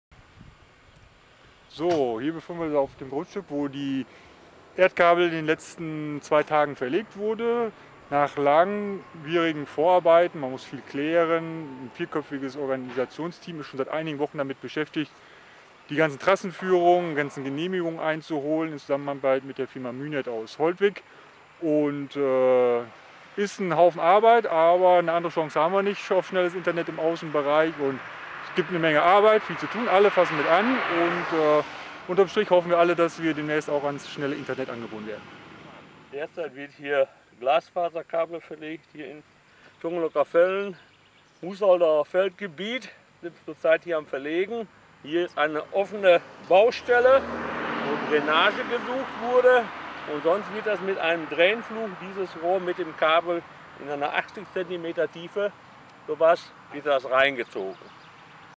Glasfaser für Tungerloh Capellen
Gescher (pd/fjk). Der erste Spatenstich für die schnelle Verbindung zur digitalen Welt ist nun auch im Außenbereich der Glockenstadt getan. Mehrere Trecker und viele helfende Hände sind heute Morgen in der Bauernschaft Tungerloh-Capellen angerückt, um dort die Pläne zur Glasfaserverlegung an diesem und in den nächsten Wochenenden in die Tat umzusetzen.

Das Planungsteam um Matthias Düchting und Markus Wellermann bringt die  nahezu unlimitierte Internetverbindung in jeden der 22 Haushalte des Planungsgebietes. Marc Füstmann, Lohnunternehmer aus Coesfeld hat einen speziellen Pflug mitgebracht mit dem die Lehrrohre und ein entsprechendes Kabelwarnband auf 90 cm Tiefe in den Boden verlegt werden. Die Tiefbauarbeiten auf den Zuwegungen von der 8,5 km langen Haupttrasse bis zu den anzuschließenden Häusern erledigen die künftigen Anschlussnehmerinnen und Anschlussnehmer in Eigenleistung.

Im Rathaus ist man erfreut. Bürgermeister Thomas Kerkhoff und Wirtschaftsförderin Heike Twyhues freuen sich über so viel Engagement der Landwirte und unterstützen das Projekt. Nur dank deren Einsatzist sei es möglich, auch die Anlieger in den Bauernschaften  kurzfristig an ein echtes Glasfasernetz anzubinden.